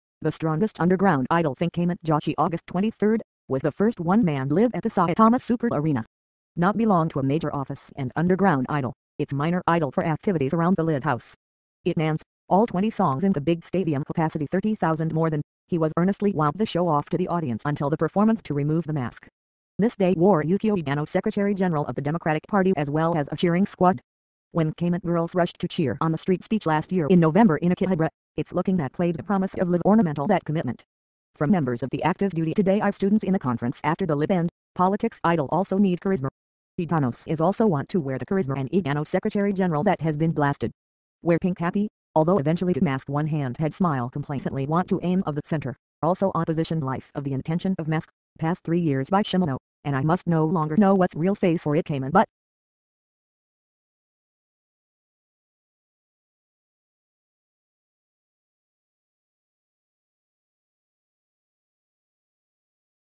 Underground Idol "Kamen Joshi" Edano said to "Give to the charisma only"
Underground Idol "Kamen Joshi" Edano or "immersed in charisma only" to Mr.? We'll explain in the video.